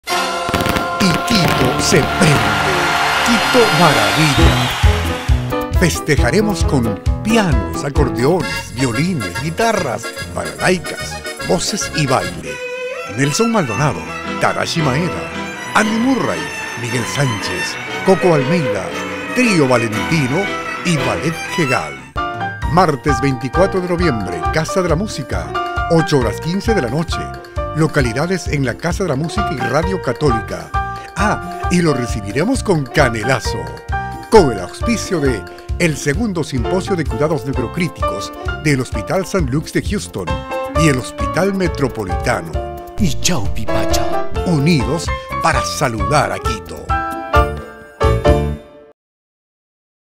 QUITO MARAVILLA
A celebrar a Quito, el 24 de Noviembre Casa de la Música Nelson Maldonado, Tadashi Maeda, Trio Valentino,Ballet Jegal, Andy Murray, Coco Almeida, Miguel Sánchez